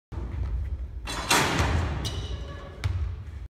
Longview's Jason Bush dunks at Azalea Orthopedics All-Star practice
Longview's Jason Bush, who will play at Tyler Junior College, dunks at Azalea Orthopedics All-Star practice Thursday at Wagstaff Gymnasium.